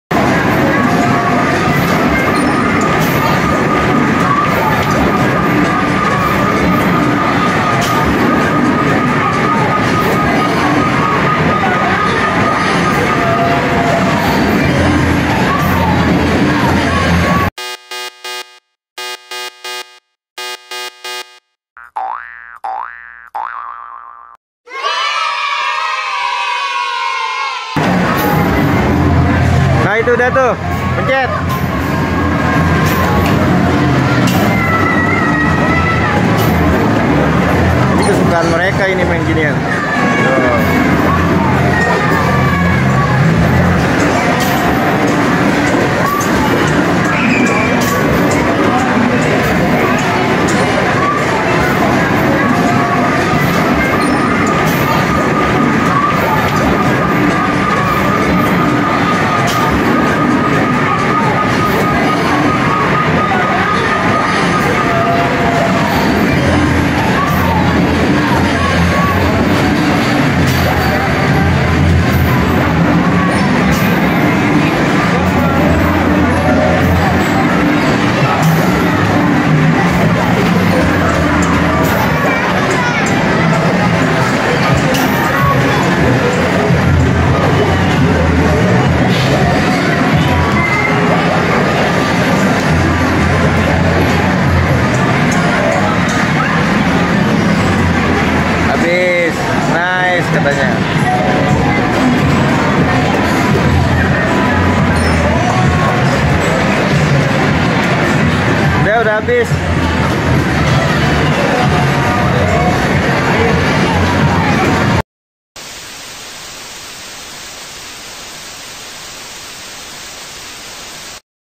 MAIN DINGDONG HUJAN BOLA DI AMAZING CARIBEAN MALL KOTA KASABLANKA
Halo Semua...

Video kali ini Arga dan Arin sedang main dingdong hujan bola di amazing caribean mall kota kasablanka